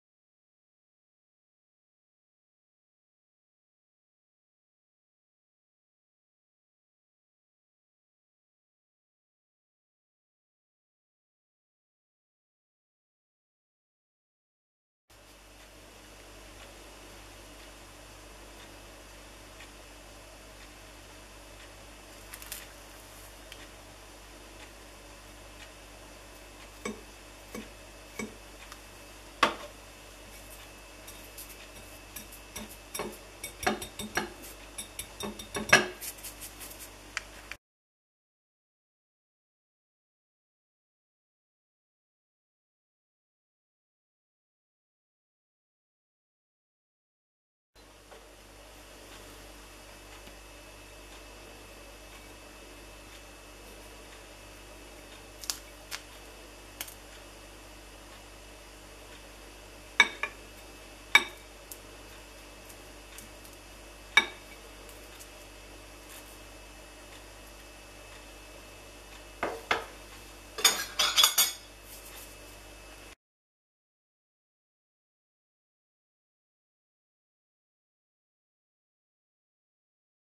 Burning metal
The unexpected Dangers of Metal Chips in the home shop